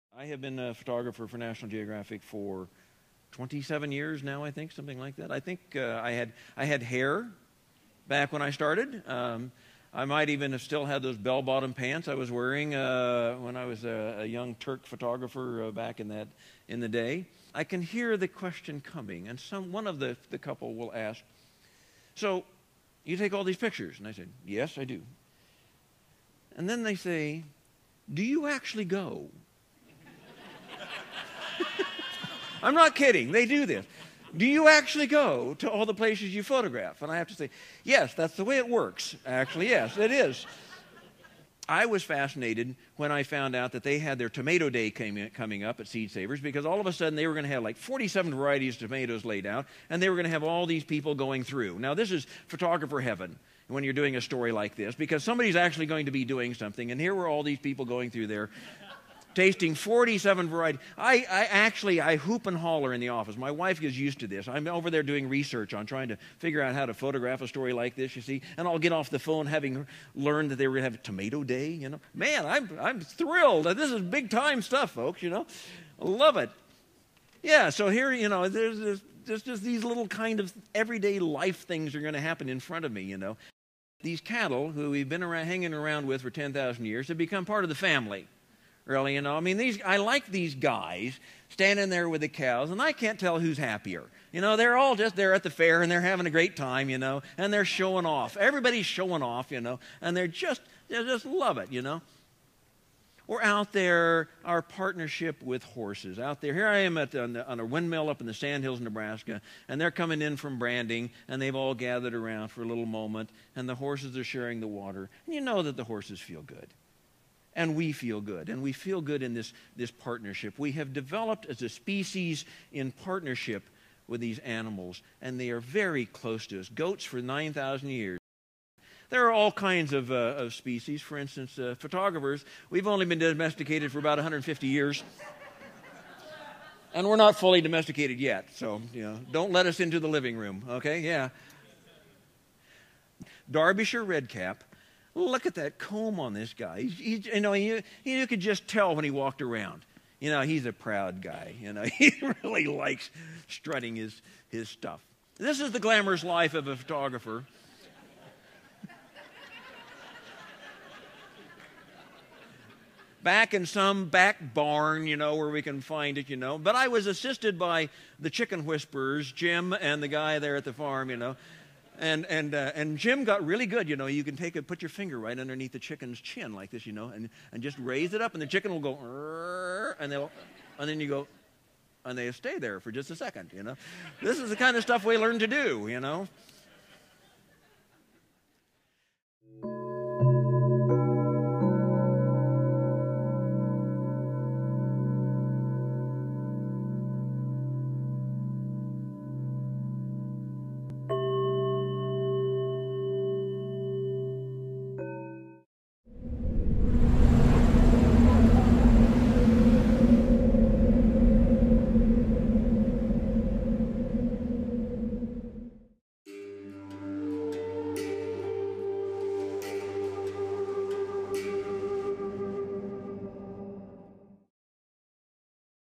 SALT Highlights: Jim Richardson
Heirlooms: Saving Humanity's 10,000-year Legacy of Food presented by Jim Richardson on February 22, 02012

Agricultural biodiversity is as much in need of defending as the world's wildlife. Countless varieties of plants and animals were bred by the world's peoples for talents specific to every soil, climate, and human culture. Most of them have been lost---their hard-won genetic sophistication extinguished. But many have survived, thanks to professional and amateur devotion, and they are wondrous---living embodiments of humanity's deepest traditions.

Photojournalist Jim Richardson has been covering the agricultural beat for National Geographic since 1984. His spectacular photographs, and the stories he tells with them, are renowned.

This highlight clip is excerpted from a lecture given as part of the Seminars About Long-term Thinking, a project of The Long Now Foundation.